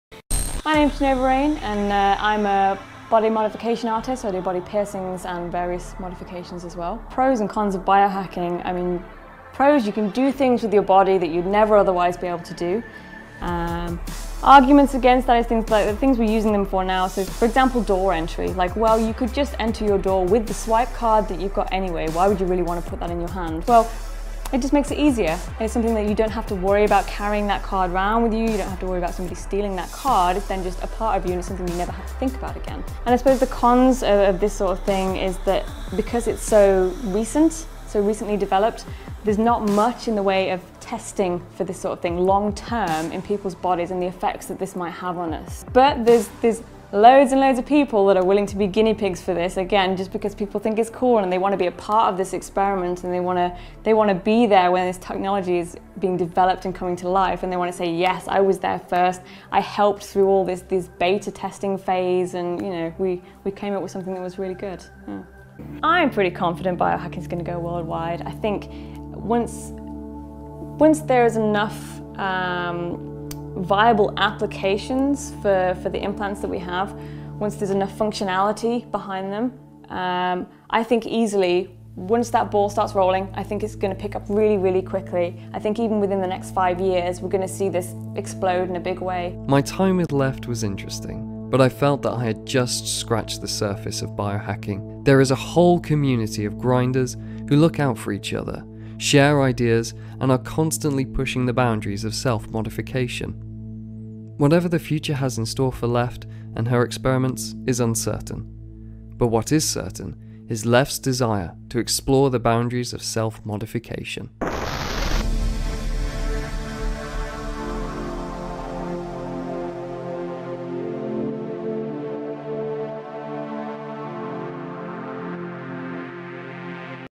Biohacking Documentary Snippets 2017 | Voxis Productions
Throwback to my contribution in the Voxis Productions' documentary made in 2017 about Lepht Anonym's work in the world of Biohacking.